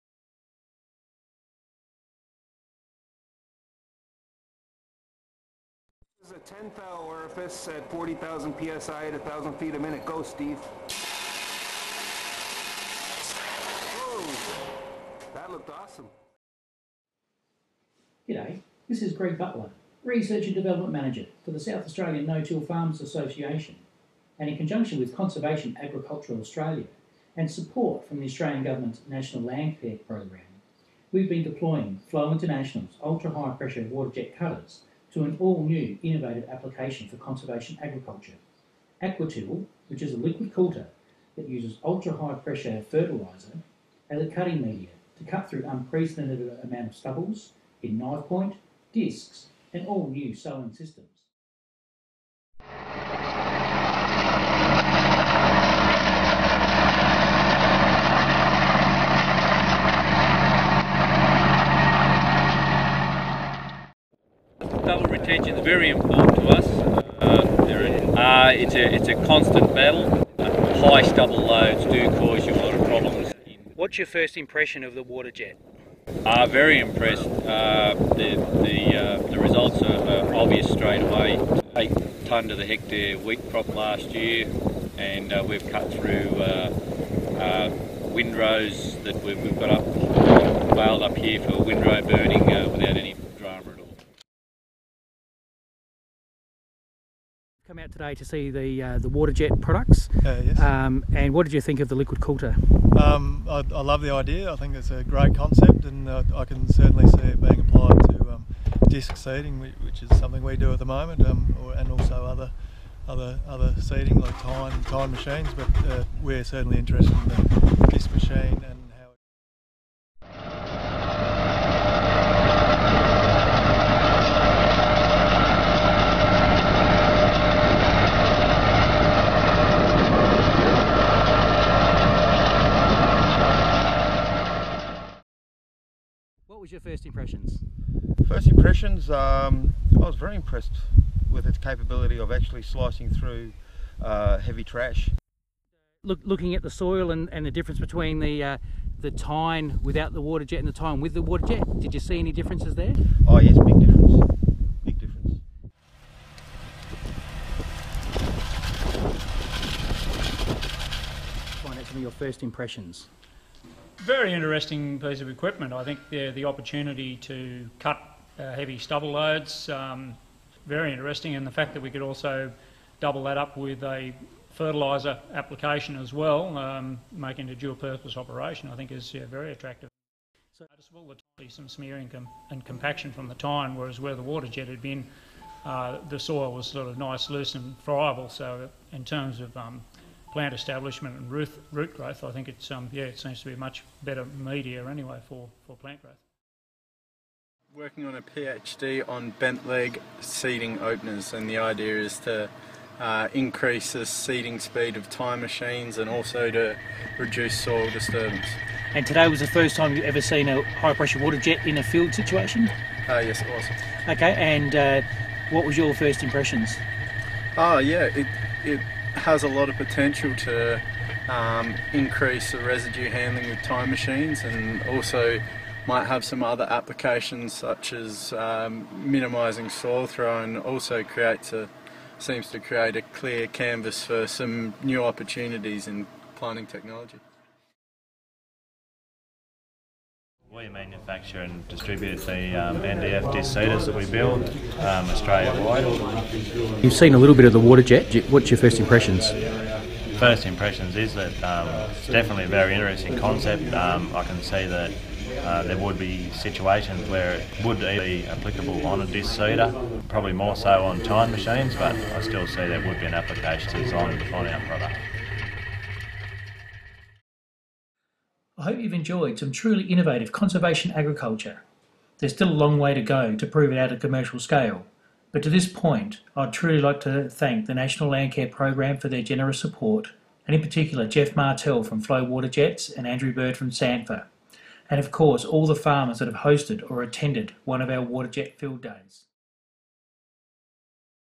Aqua Till Concept Demonstrations 2015 Movie
Aqua-Till employs FLOW high-pressure water-jet cutting tools as a liquid coulter.   The Aqua-Till concept demonstrations are supported by the Australian Federal Government National Landcare Program.

An overview including modelling of opex and liquid rates will be provided at the SANTFA Conference in South Australia in February 2016.

We will also be at the "No-Till on the Plains Conference" in Kansas during late January 2016.  We'll be talking at the Morning Marketplace.

Cheers

Greg Butler
R&D Manager, SA No-Till Farmers Association